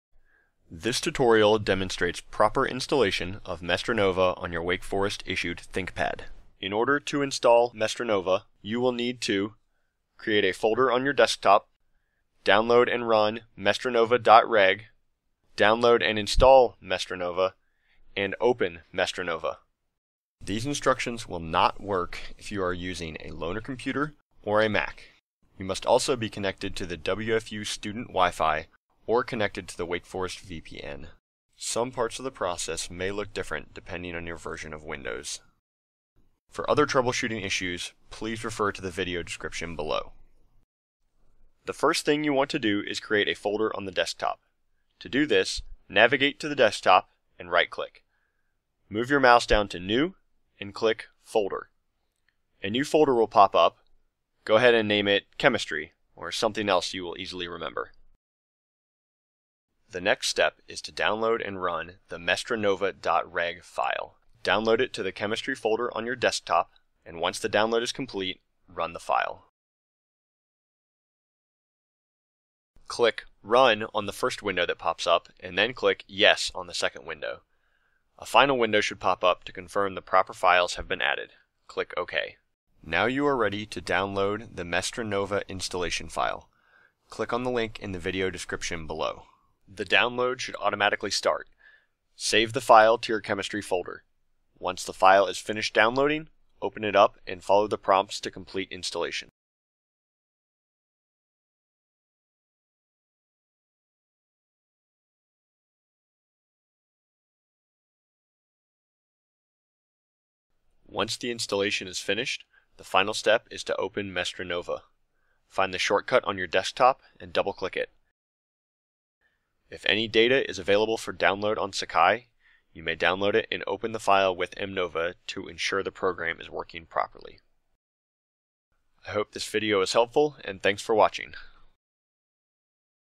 Installing MestReNova
For more information about the chemistry department at Wake Forest, go

This tutorial will NOT work if you are using a loaner or a mac.

If the MestReNova.reg file produces an error, right-click on the file and click "run as administrator".

Produced and edited by Jason Pifer.